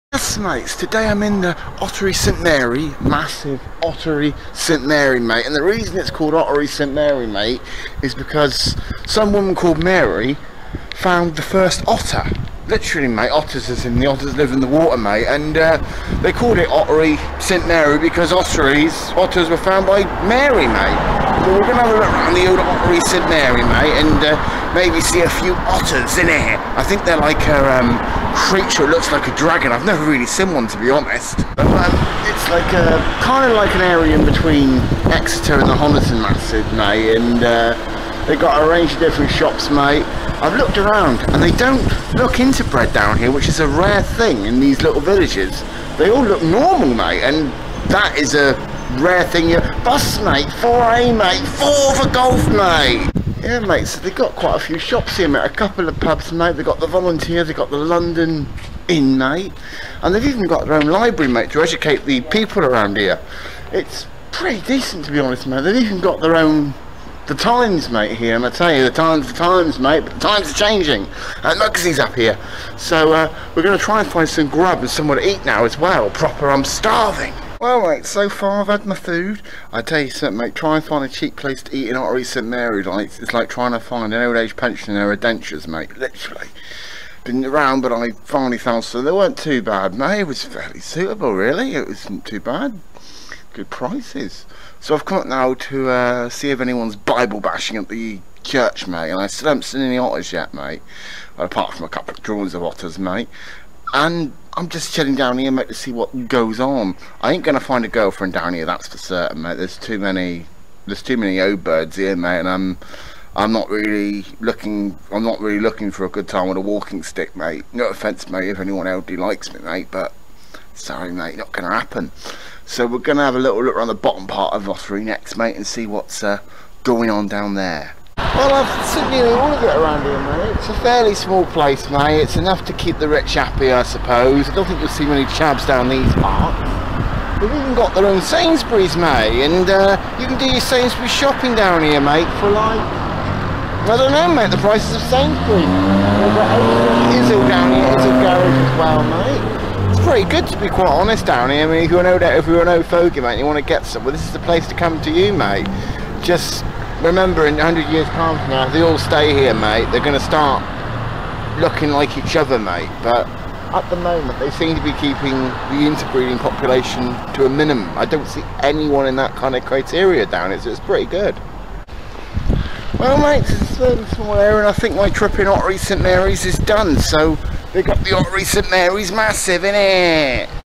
Ottery St Mary Massive
Today my destination was Ottery St Mary Massive a small town which can be found between Exeter and Honiton. Why actually a small town there is actually an intelligent civilians dwelling in this town that understand not to breed with family members a rare find in a small town making this little place a hidden gem. Equipped with a superstore in the form of the colour orange and a nice place to take the grandparents if they enter expensive food that requires a good pension. The town is also famous and this is where people discovered what is known as Otters who get named after a women called Mary Otter.